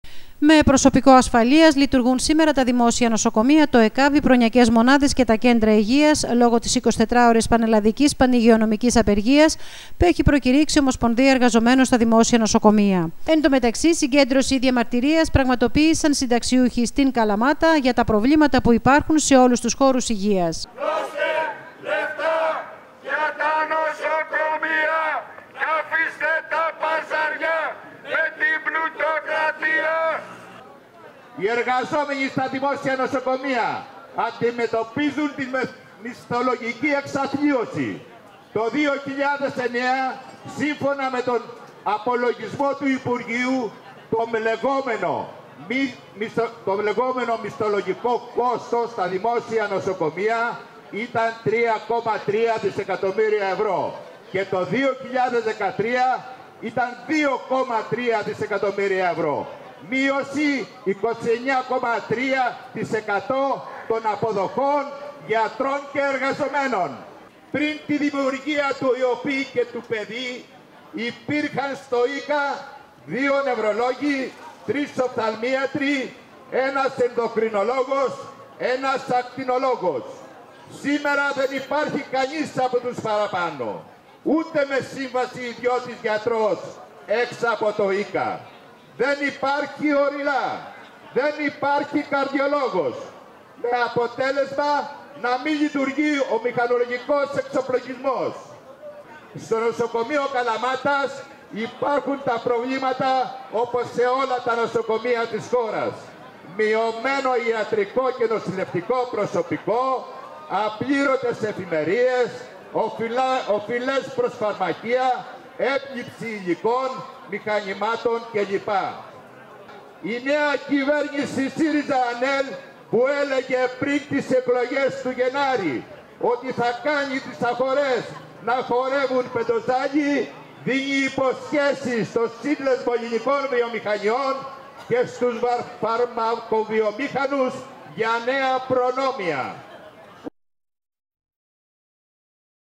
SYNTAKSIOUXOI
24ωρη απεργία πραγματοποιούν σήμερα  οι γιατροί και οι εργαζόμενοι στα Δημόσια Νοσοκομεία διαμαρτυρόμενοι μεταξύ άλλων για τις απλήρωτες εφημερίες ενώ η Ένωση Νοσοκομειακών Γιατρών Ελλάδας, ζητά άμεσα μέτρα ενίσχυσης των δημόσιων νοσοκομείων και Κέντρων Υγείας με μόνιμο προσωπικό και ισχυρή χρηματοδότηση από τον κρατικό προϋπολογισμό. Συγκέντρωση διαμαρτυρίας όμως για τα προβλήματα στο χώρο της υγείας πραγματοποίησαν σήμερα συνταξιούχοιστην Καλαμάτα.